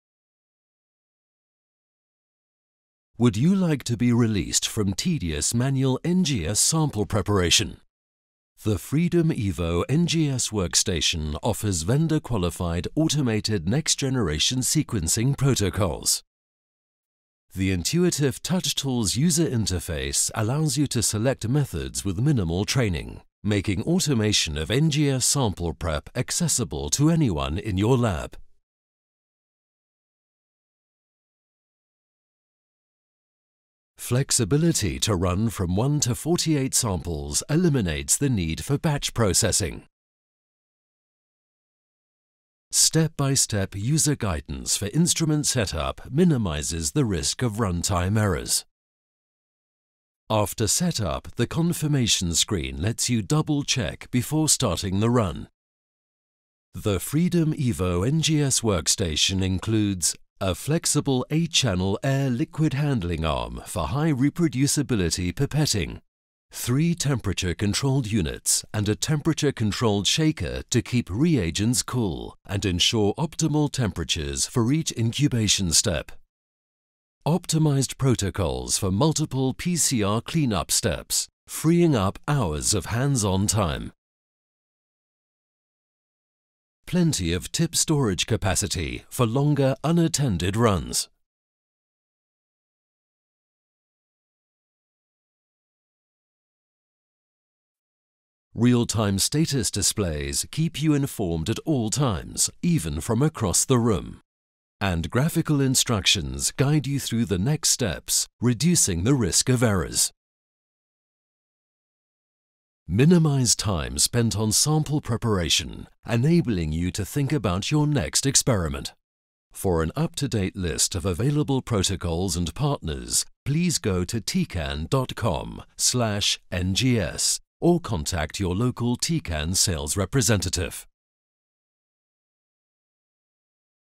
Freedom EVO NGS workstation - Automated NGS sample preparation
Next generation sequencing (NGS) technology is advancing at an unprecedented speed. In conjunction with the ever increasing throughput of NGS instruments and the option to multiplex more and more samples, sample preparation has become a bottleneck in many laboratories.

Automation of NGS sample preparation on the Tecan Freedom EVO NGS workstation increases throughput, reproducibility and guides users through each step of protocol selection and worktable set-up with the TouchTools operator interface.

Tecan is developing and optimizing automated NGS sample preparation protocols in collaboration with key research centres, partners including Illumina ® and other NGS sample preparation kit vendors to qualify and test the protocols to ensure that they work in your laboratory.